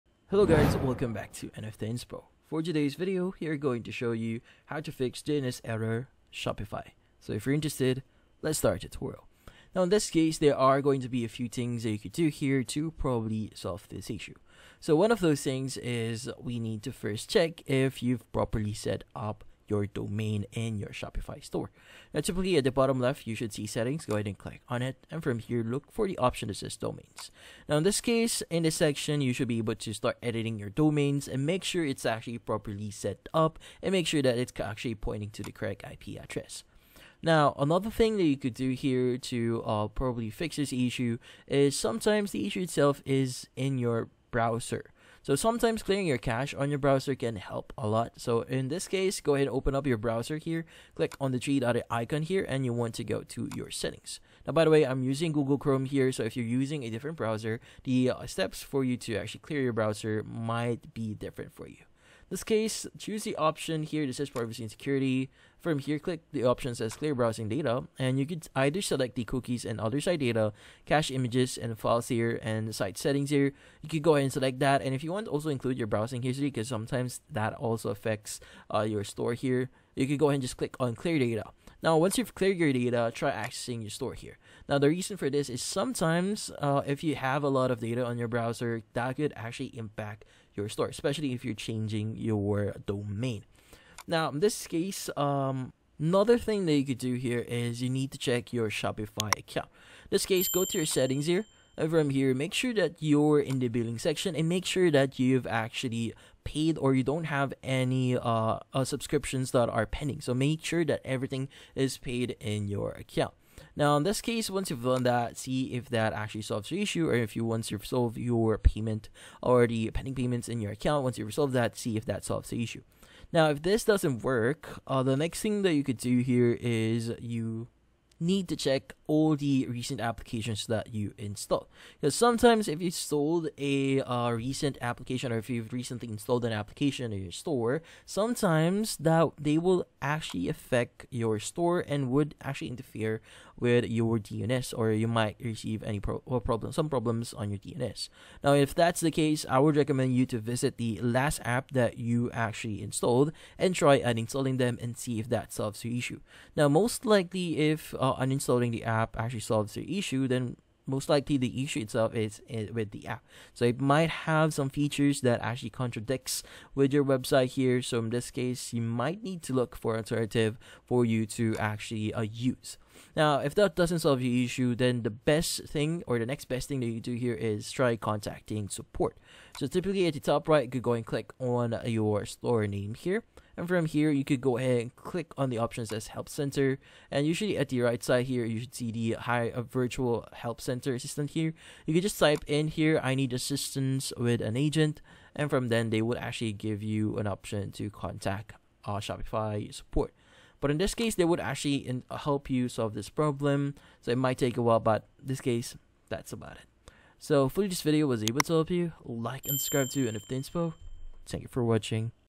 How To Fix DNS Error Shopify (2025)
If you're encountering a DNS error on Shopify and need a fix, then this video will be perfect for you!